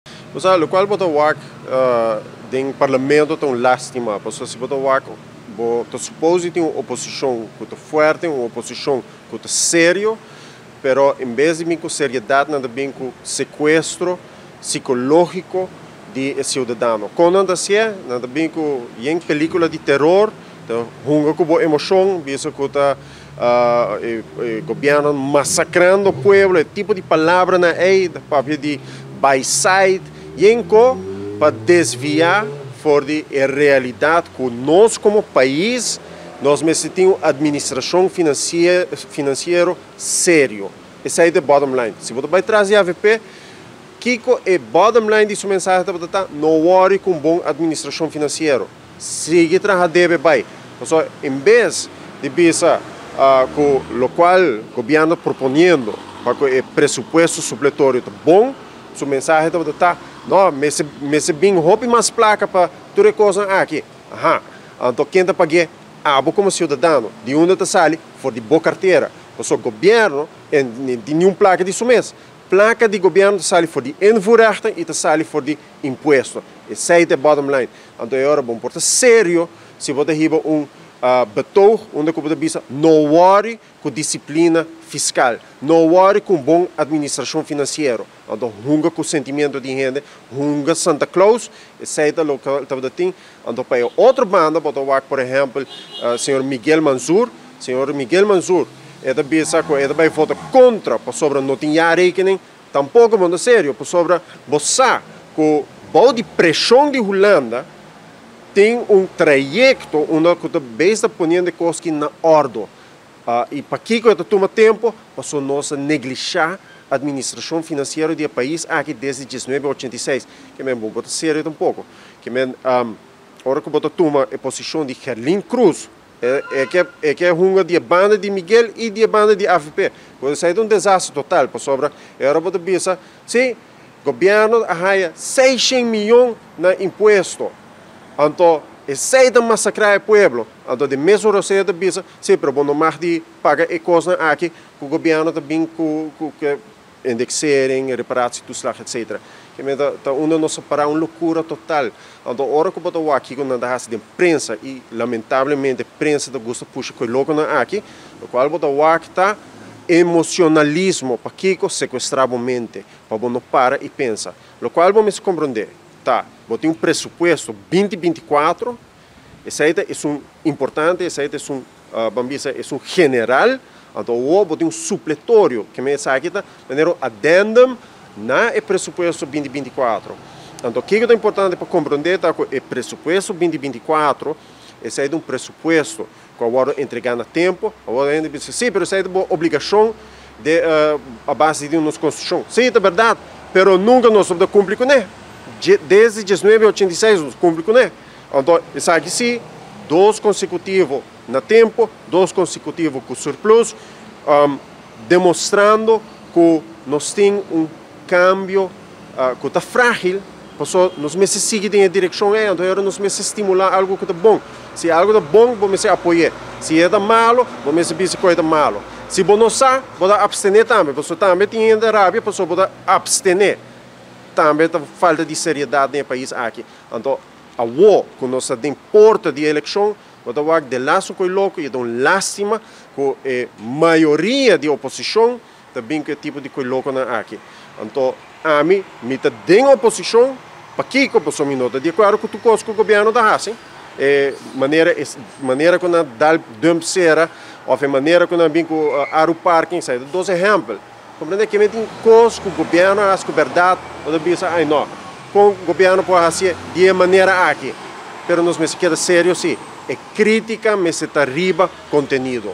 No ta un secreto dicon Aruba no tin un jaarrekening na ordo, parlamento completo a haya informacion
Parlamentario independiente Ryçond Santos do Nascimento ta expresa cu loke cu por mira den parlamento ta un lastima. Den parlamento lo mester tin un oposicion cu ta fuerte y serio , pero enbes di bin cu seriedad ta bin cu secuestro psicologico di e ciudadano.